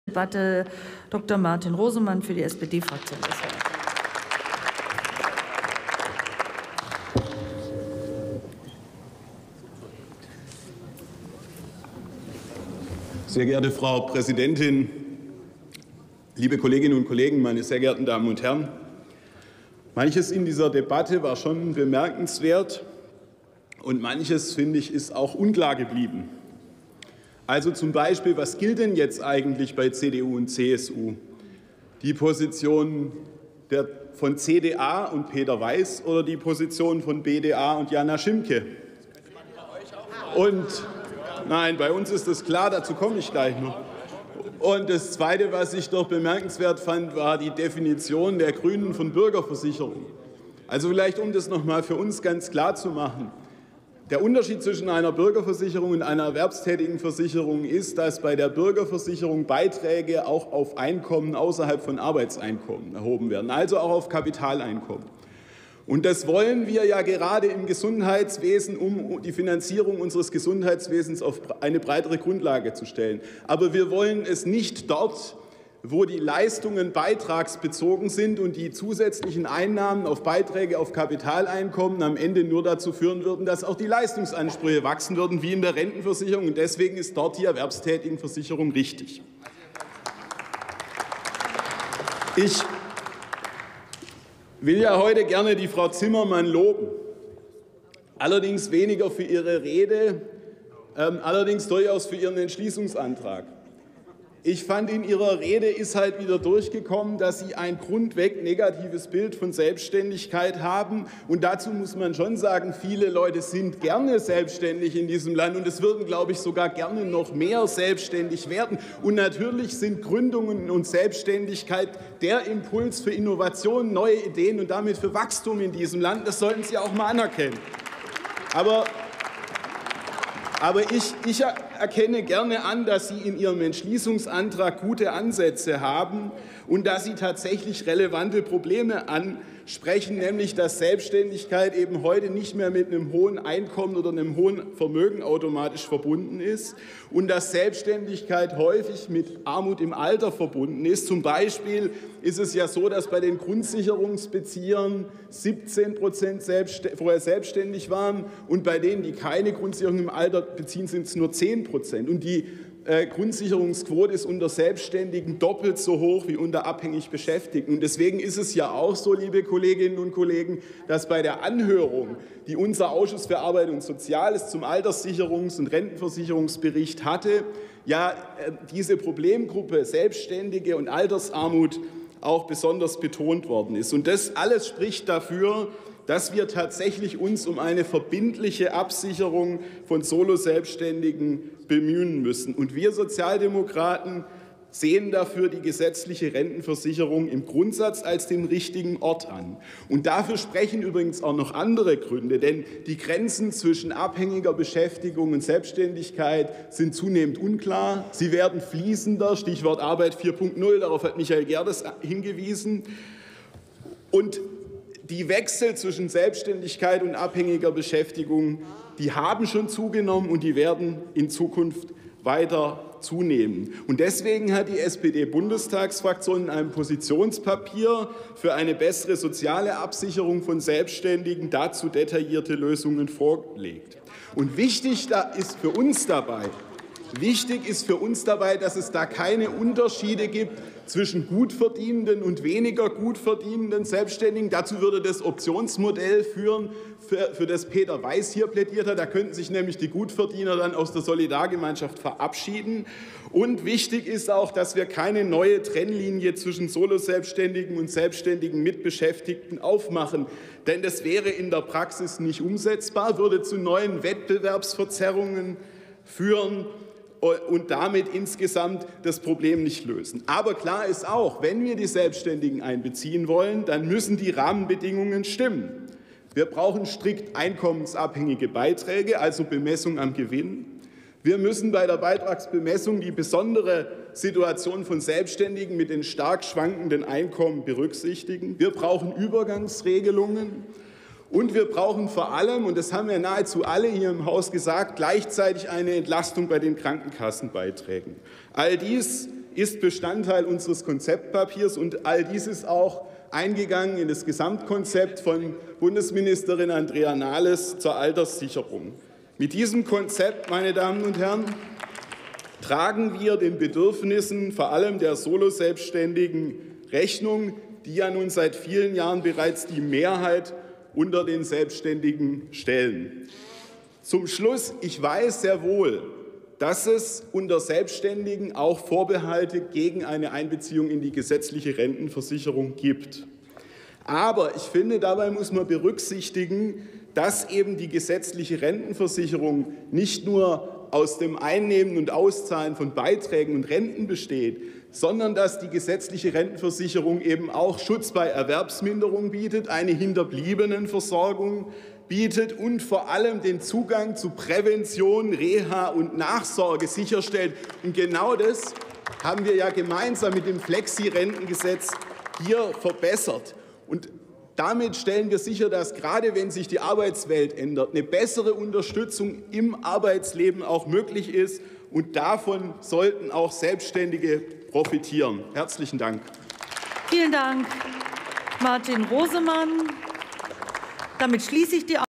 Martin Rosemann: Soziale Absicherung von Solo-Selbständigen [Bundestag 17.02.2017]
Beratung der Antwort der Bundesregierung
auf die Große Anfrage der Fraktion DIE LINKE.
Soziale Lage und Absicherung von
Solo-Selbstständigen

18. Deutscher Bundestag
219. Plenarsitzung
Tagesordnungspunkt: Soziale Absicherung von Solo-Selbständigen
Redner: Rosemann, Dr. Martin, SPD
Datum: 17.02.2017
Plenarprotokoll:
UVID:7074358